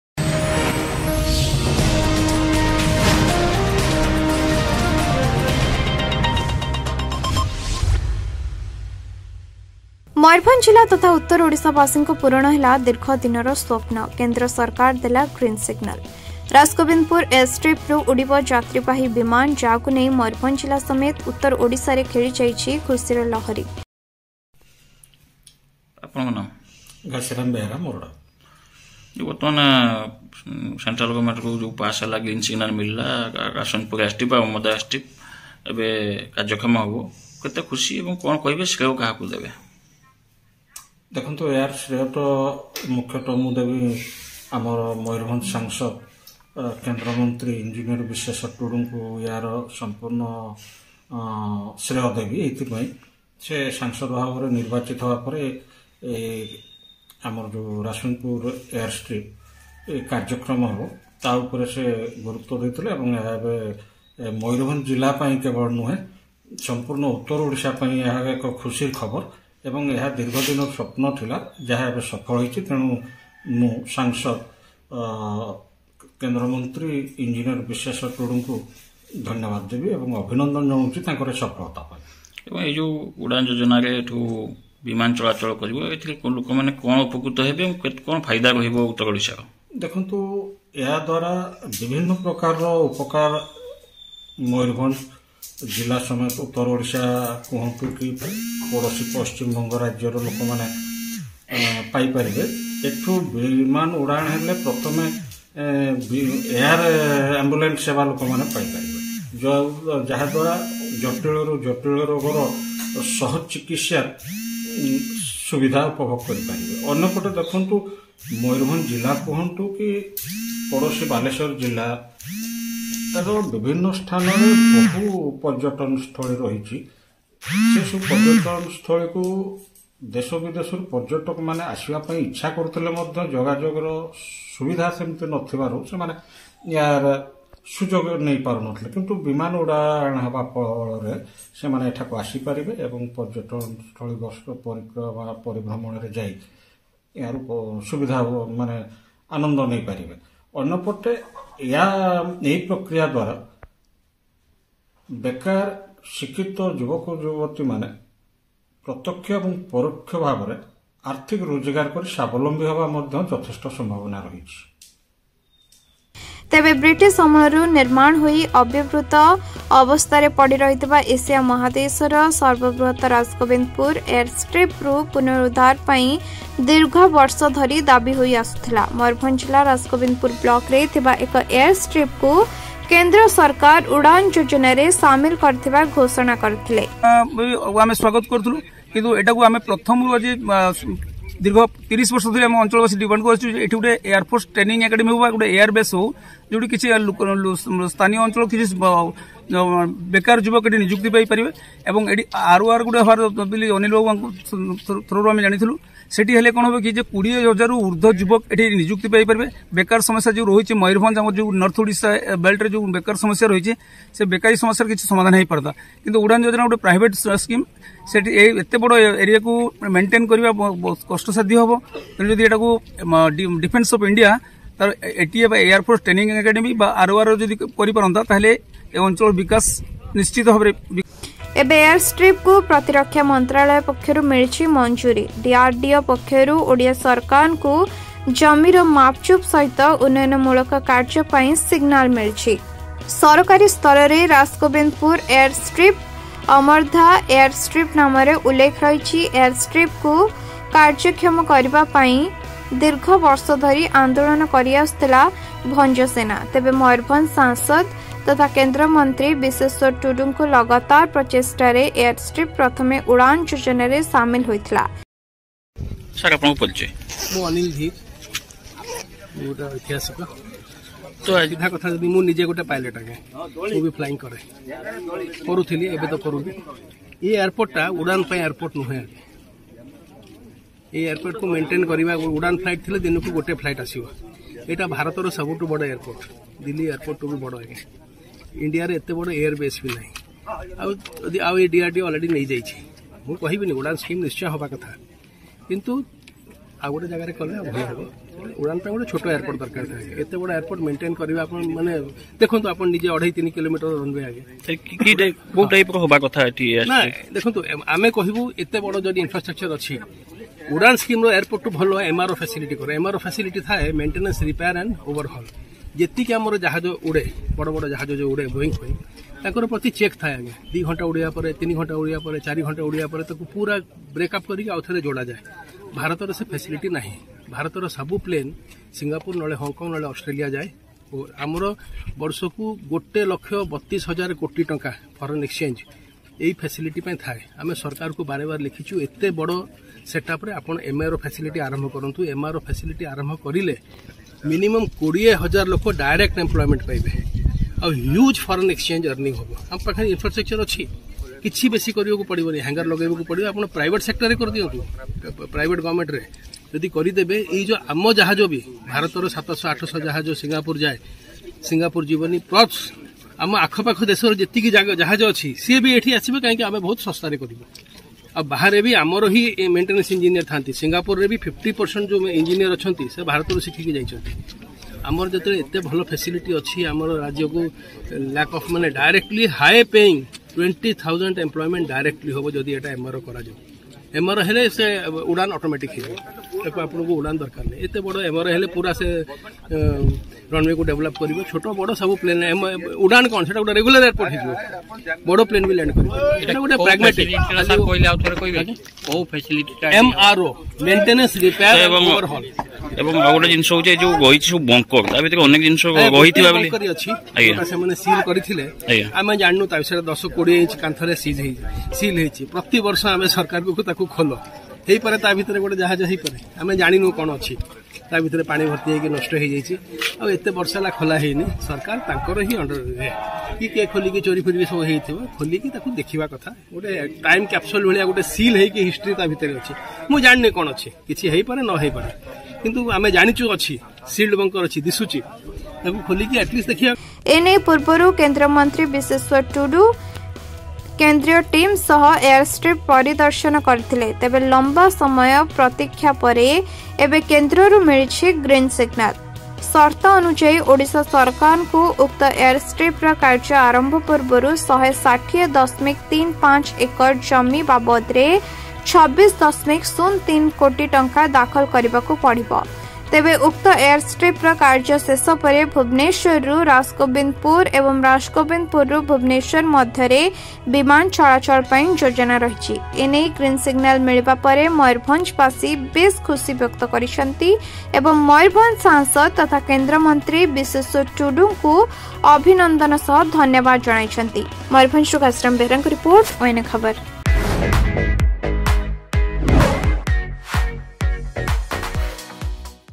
Mayurbhanj: Passenger Flights will Fly from Rasgobindpur Airstrip
About Channel:
ଚ୍ୟାନେଲ୍ ବିଷୟରେ.